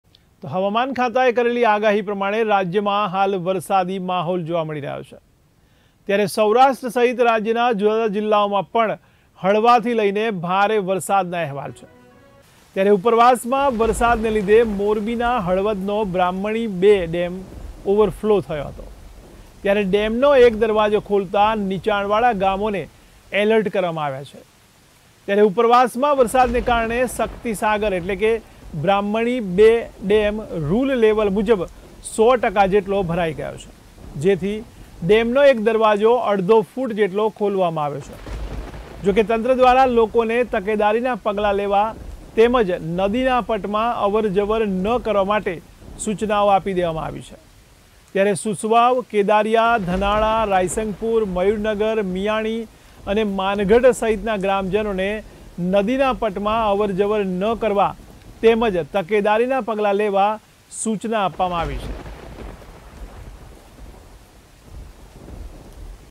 મોરબીના હળવદનો બ્રાહ્મણી-૨ ડેમ ઓવરફ્લો
મોરબીના હળવદનો બ્રાહ્મણી-૨ ડેમ ઓવરફ્લો , Brahmani-2 dam overflow of Morbi's Halwad
ડેમનો એક દરવાજો ખોલતા નિચાણવાળા ગામોને એલર્ટ
બ્રાહ્મણી-૨ ડેમ રૂલ લેવલ મુજબ 100% ભરાયો
ડેમનો એક દરવાજો અડધો ફૂટ ખોલવામાં આવ્યો
નદીના પટમાં અવર-જવર ન કરવા સૂચના